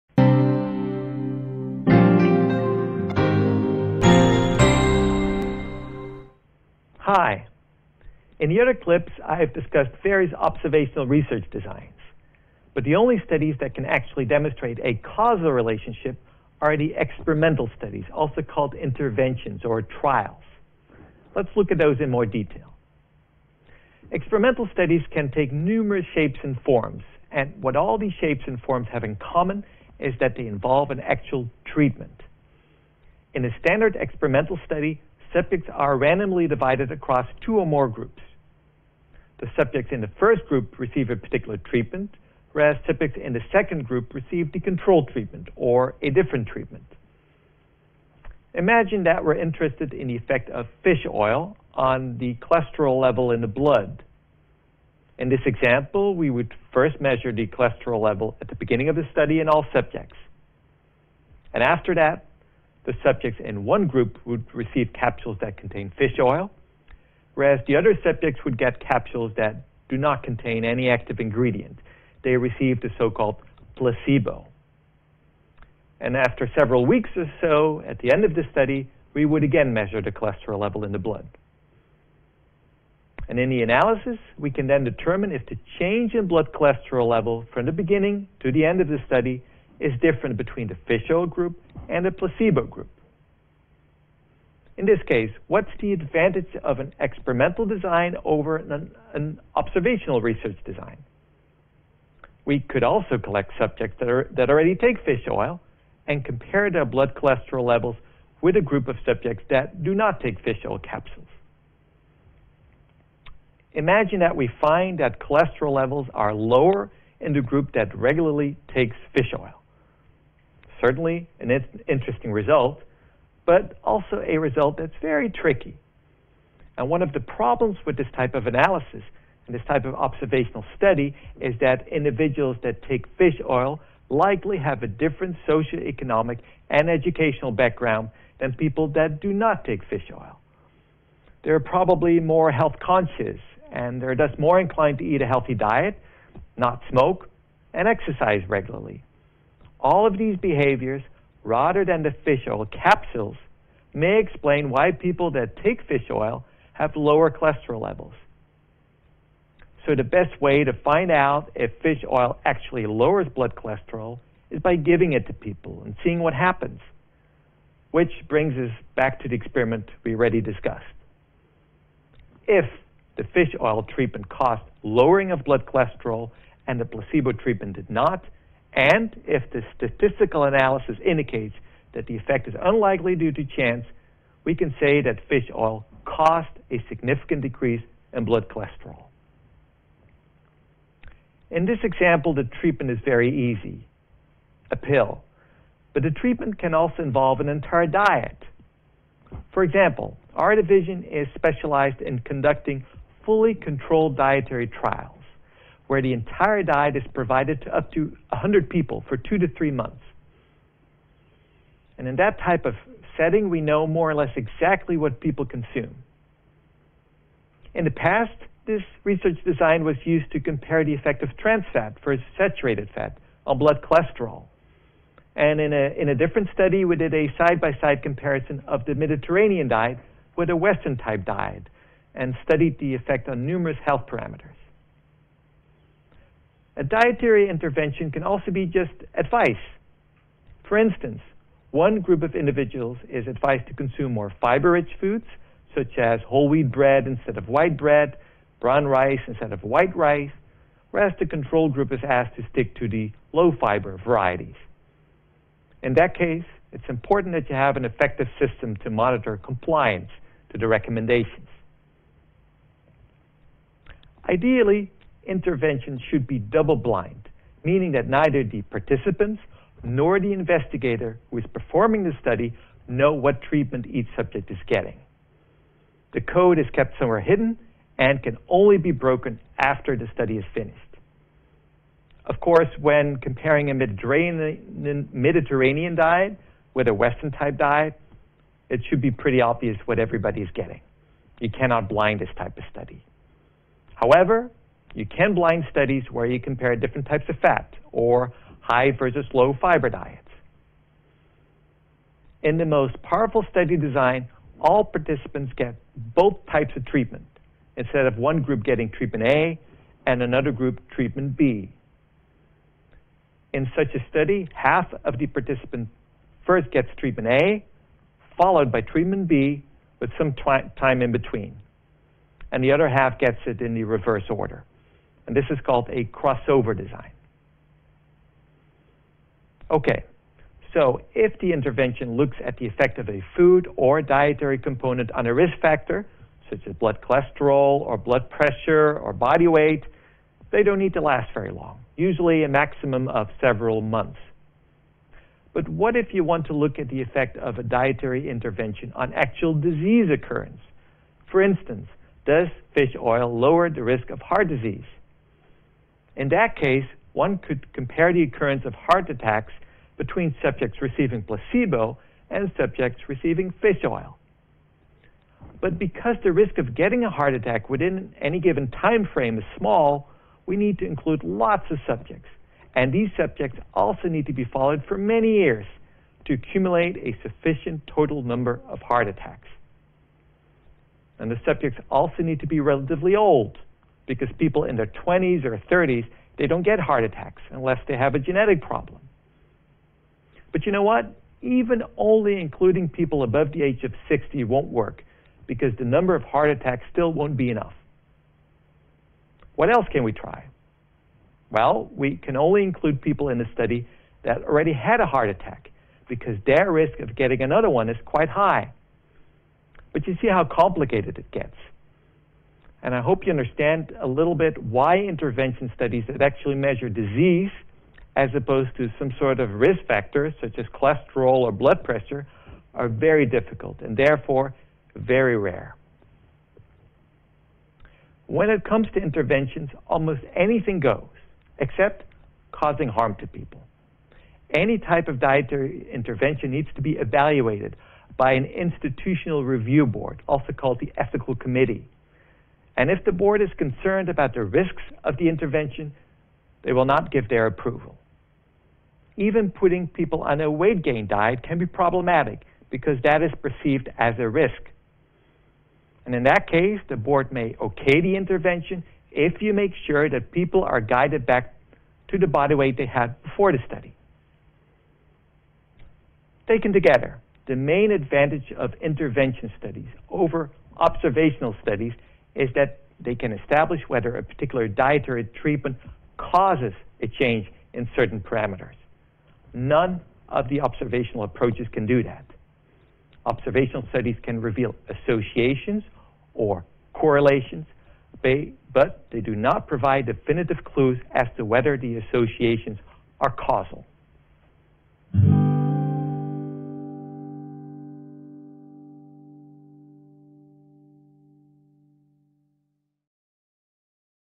1.2.7 Randomized Controlled Trials | 1.2.7 Thử nghiệm ngẫu nhiên có kiểm soát
1.2.7 Thử nghiệm ngẫu nhiên có kiểm soát
Các thử nghiệm ngẫu nhiên có kiểm soát
Trong video tiếp theo, giáo sư Sander Kersten sẽ giải thích cho bạn những lợi ích và sự phức tạp của thiết kế nghiên cứu cuối cùng mà chúng tôi muốn giới thiệu với bạn: các thử nghiệm ngẫu nhiên có kiểm soát, là các nghiên cứu thử nghiệm.